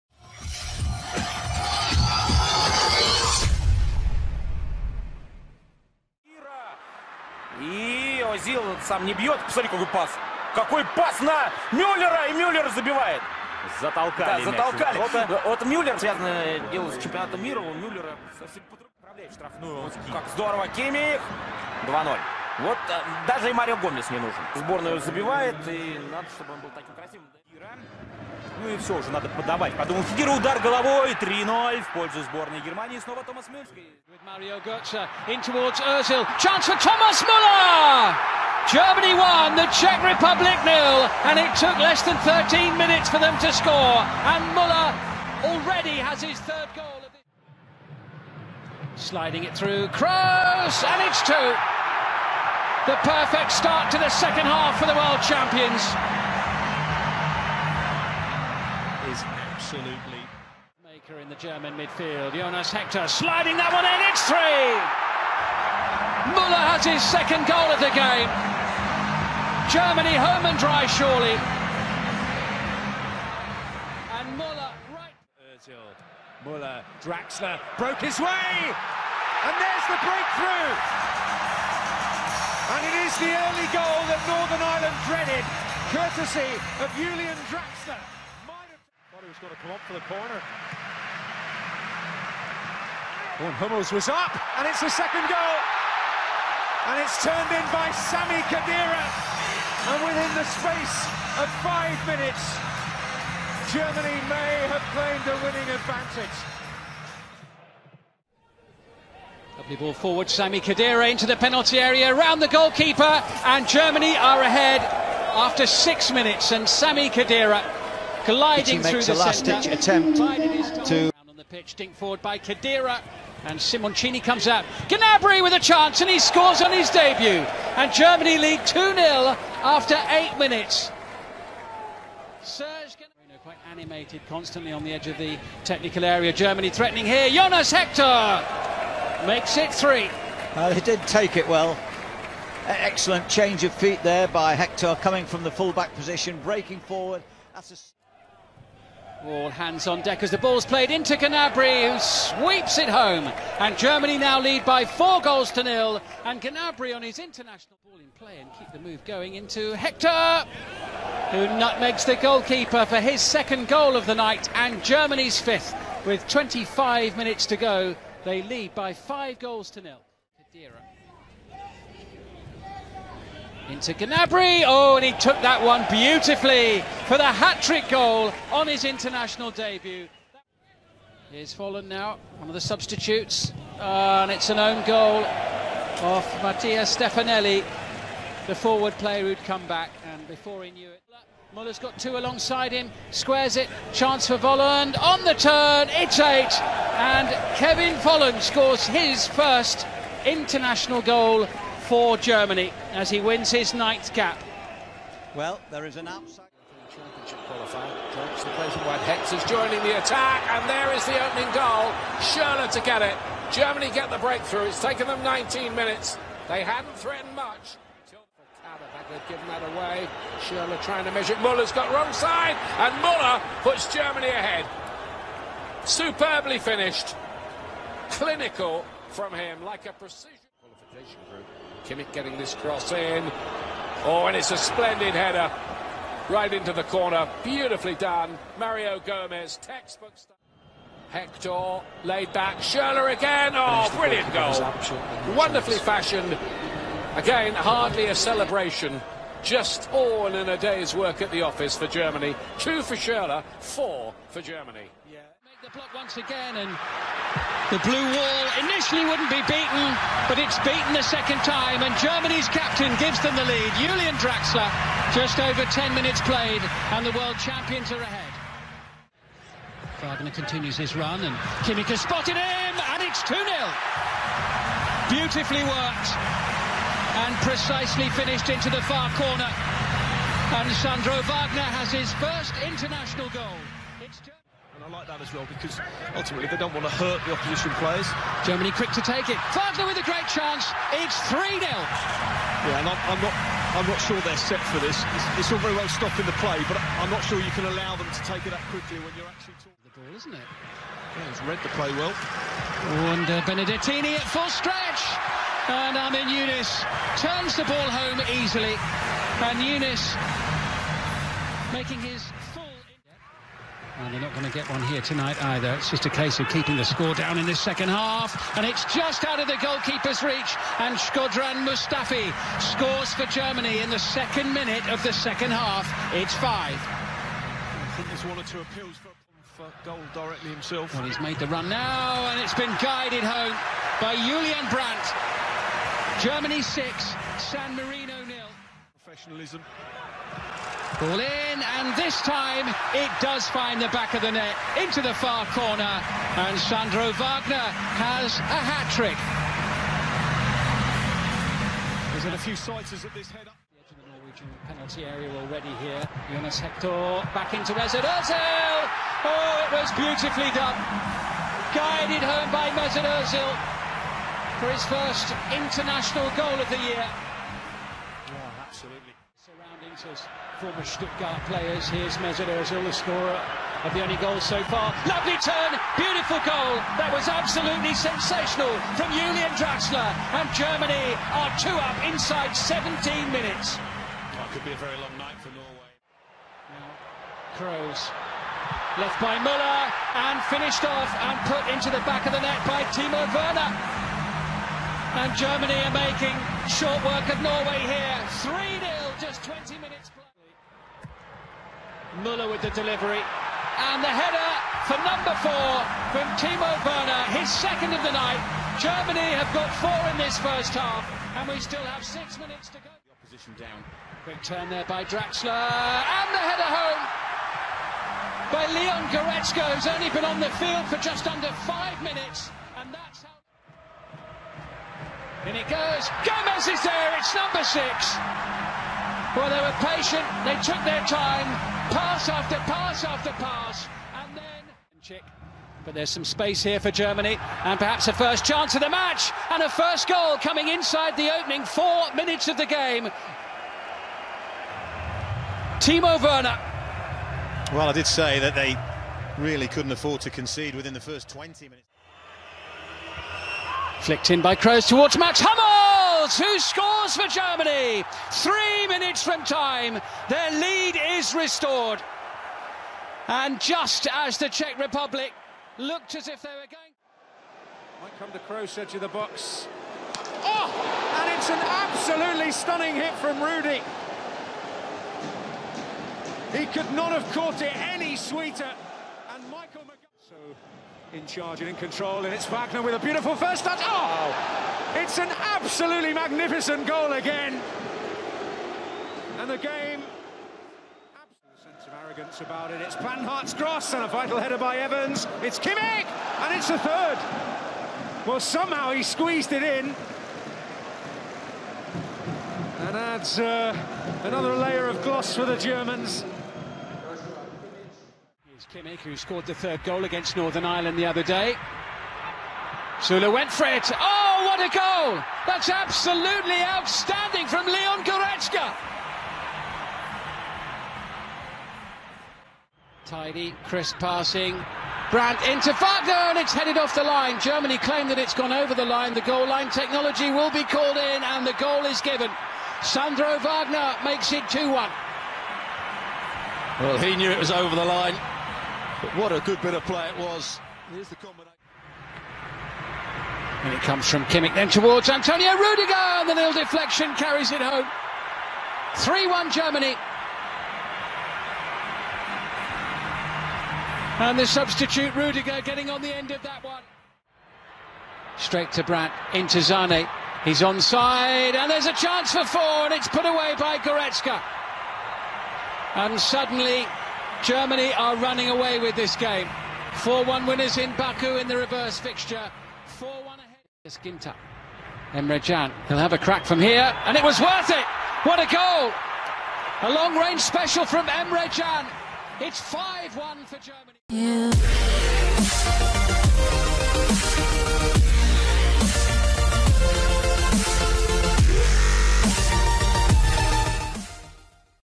Germany ● Road to Russia● All 43 goals in World Cup 2018 Qualifiers European
☑️ see more "All goals in the 2018 World Cup qualifiers of the teams"
☑️ Road to Russia to Germany. All 43 goals in World Cup 2018 Qualifiers European.
☑️ Matches played of Germany:
Match 1: Norway 0 - 3 Germany
1 Thomas Muller
2 Joshua Kimmich
3 Thomas Muller
Match 2: Germany 3 - 0 Czech Republic
4 Thomas Muller
5 Toni Kroos
6 Thomas Muller
Match 3: Germany 2 - 0 Northern Ireland
7 Julian Draxler
8 Sami Khedira
Match 4: San Marino 0 - 8 Germany
9 Sami Khedira
10 Serge Gnabry
11 Jonas Hector
12 Serge Gnabry
13 Jonas Hector
14 Serge Gnabry
15 Mattia Stefanelli (og)
16 Kevin Volland
Match 5: Azerbaijan 1 - 4 Germany
17 Andre Schurrle
18 Thomas Muller
19 Mario Gomez
20 Andre Schurrle
Match 6: Germany 7 - 0 San Marino
21 Julian Draxler
22 Sandro Wagner
23 Sandro Wagner
24 Amin Younes
25 Shkodran Mustafi
26 Julian Brandt
27 Sandro Wagner
Match 7: Germany 6 - 0 Norway
28 Mesut Ozil
29 Julian Draxler
30 Timo Werner
31 Timo Werner
32 Leon Goretzka
33 Mario Gomez
Match 8: Czech Republic 1 - 2 Germany
34 Timo Werner
35 Mats Hummels
Match 9: Northern Ireland 1 - 3 Germany
36 Sebastian Rudy
37 Sandro Wagner
38 Joshua Kimmich
Match 10: Germany 5 - 1 Azerbaijan
39 Leon Goretzka
40 Sandro Wagner
41 Antonio Rudiger
42 Leon Goretzka
43 Emre Can